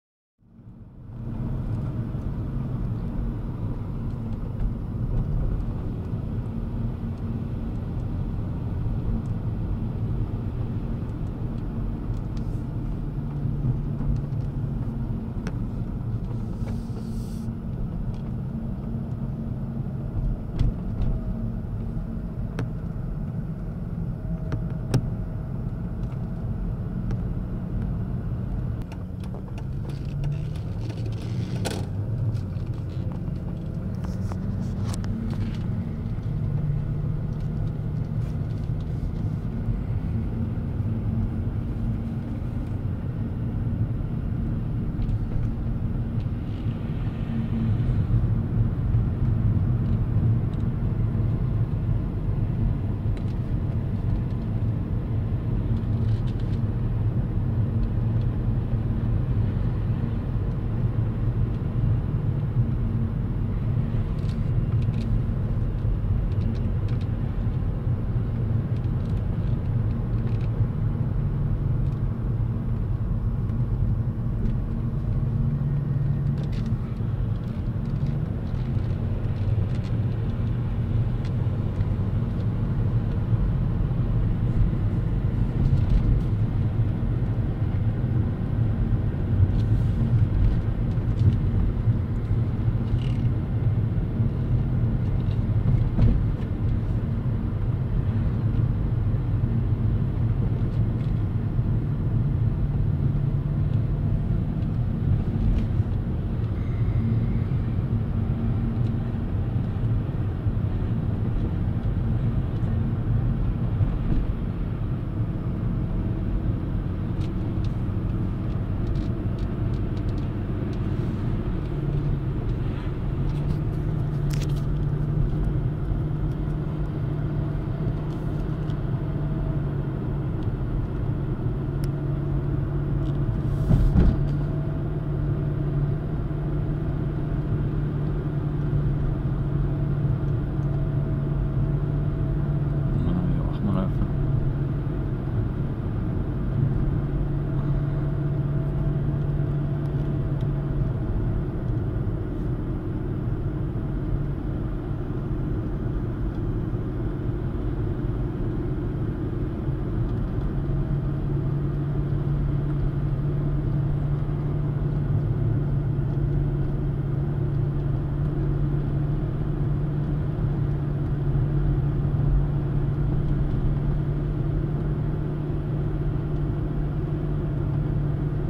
Aerdenhout, Overveen en Bloemendaal in herfsttinten, deel 3.
Een video al rijdend gemaakt (door de bijrijder) langs de Brouwerskolk en vervolgens langs Duinlust en Kraantje Lek d.d. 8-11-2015 om 8.53 s'ochtends.